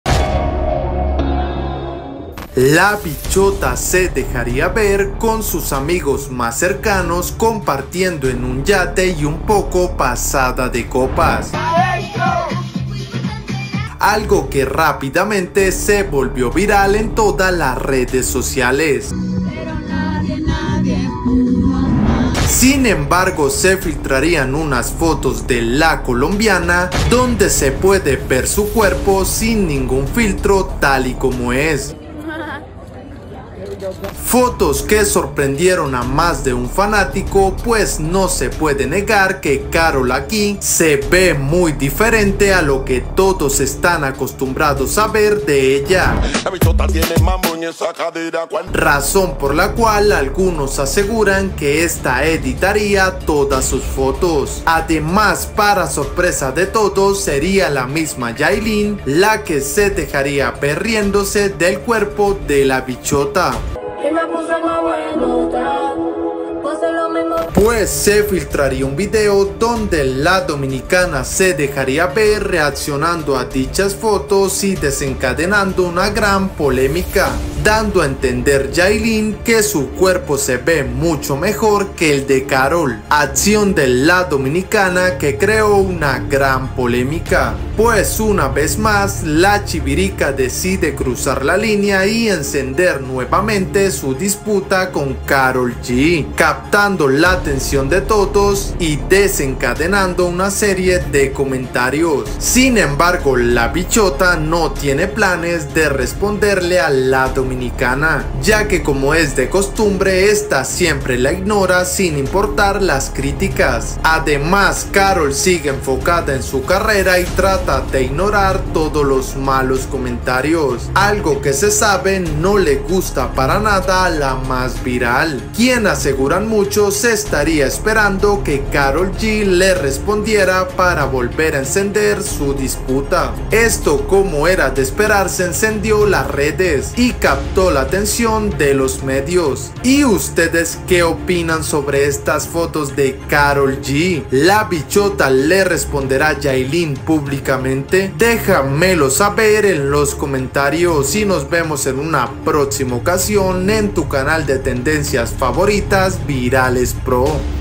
Karol G Enseña Su Cuerpo Sin Filtro Y Yailin Se Burla En Su Cara
Karol g seria captada en las ultimas horas con sus amigos mas cercanos y enseñaría su cuerpo sin filtro a todos sus fanáticos, razón por la cual la bichota seria criticada, sin embargo yailin la mas viral reaccionaria a esto y se burlaría de dichas fotos